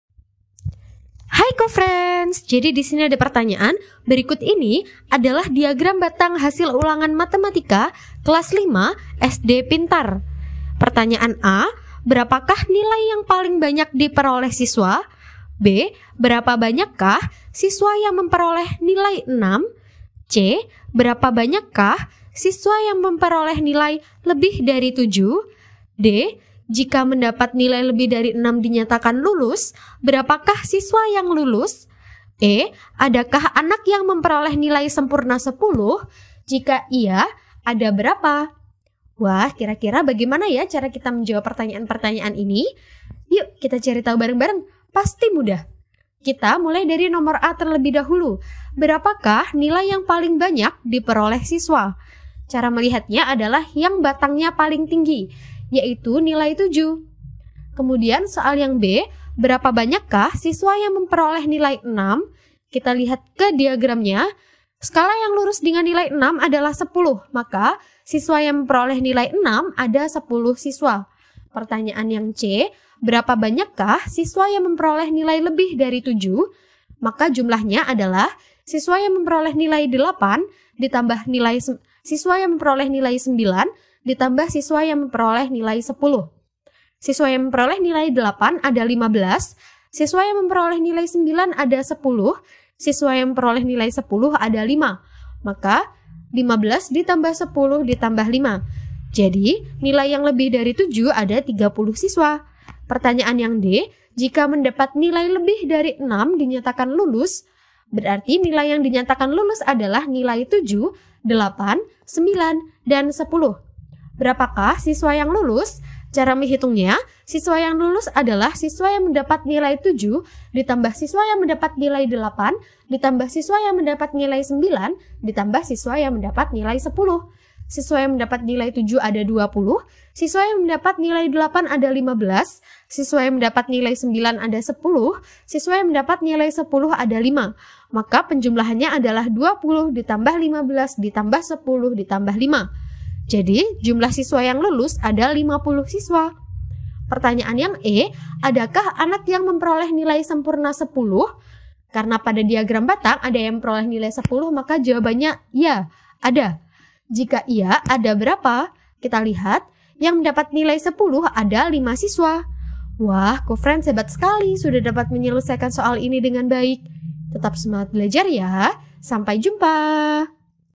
Berikut ini adalah diagram batang hasil ulangan Matematika kelas V SD Pintar: 25 20 1 15 10 10 Ni...
Berikut ini adalah diagram batang hasil ulangan Matematika kelas V SD Pintar: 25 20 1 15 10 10 Nilai Gambar 7.10 Diagram batang Berapakah nilai yang paling banyak diperoleh siswa? b. Berapa banyakkah siswa yang memperoleh nilai 62 Berapa banyakkah siswa yang memperoleh nilai lebih dari 7? d Jika mendapat nilai lebih dari 6 dinyatakan lulus; berapakah siswa yang lulus? Adakah anak yang memperoleh nilai sempurna 10? Jika ya, ada berapa?

Topik atau Materi: Penyajian Data dalam Bentuk Diagram Batang - Pengumpulan dan Penyajian Data - Statistika - Matematika Kelas 5 SD - K-13 Revisi

Sumber Soal: Dunia Matematika Kelas 5, K-13 Revisi, IAT, 2020 , Tiga Serangkai

00:00 : Bedah Soal
01:00 : Konsep, rumus dan pengertian Penyajian Data dalam Bentuk Diagram Batang
01:18 : Langkah penyelesaian soal
01:50 : Jawaban akhir
02:20 : Penutup